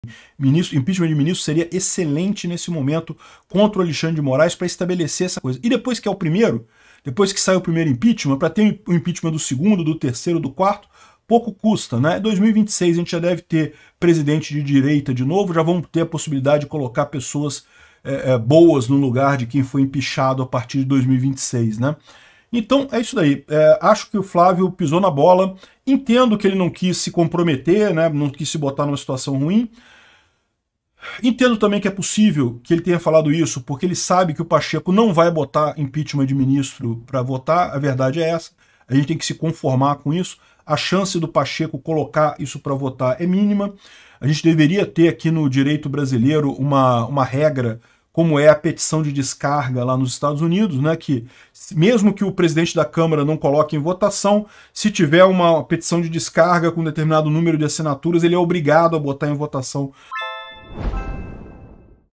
O impeachment de ministro: uma análise política
O vídeo discute a possibilidade de impeachment do ministro Alexandre de Moraes como uma estratégia política no atual cenário. O orador acredita que, uma vez estabelecido o primeiro impeachment, os próximos seriam mais fáceis de serem realizados. Ele prevê a eleição de um presidente de direita em 2026 e a substituição de pessoas consideradas inadequadas. O orador critica a postura de Flávio, sugerindo que ele não quis se comprometer. No geral, o vídeo aborda a questão do impeachment como uma forma de mudança política no Brasil.